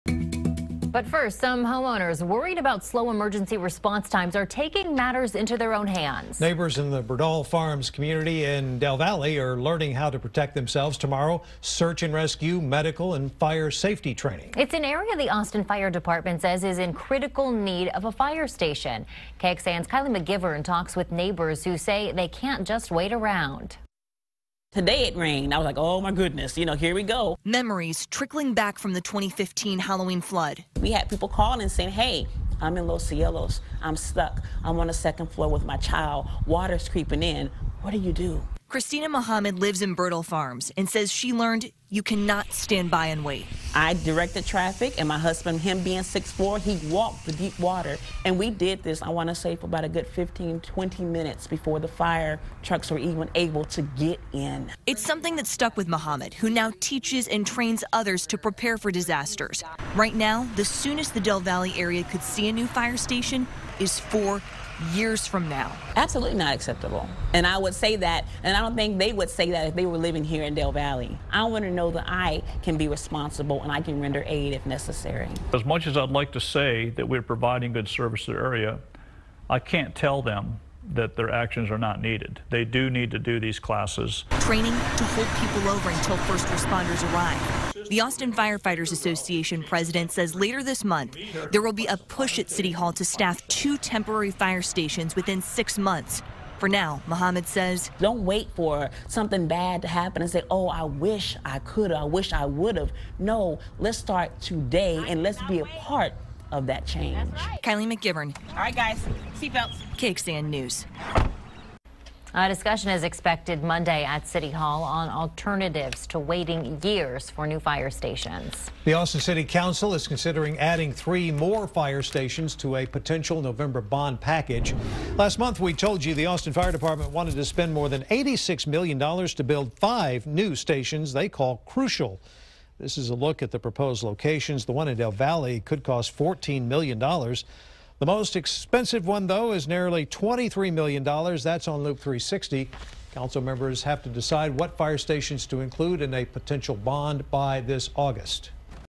Del Valle homeowners worried about slow emergency response
Neighbors in the Berdoll Farms Community in Del Valle are learning how to protect themselves.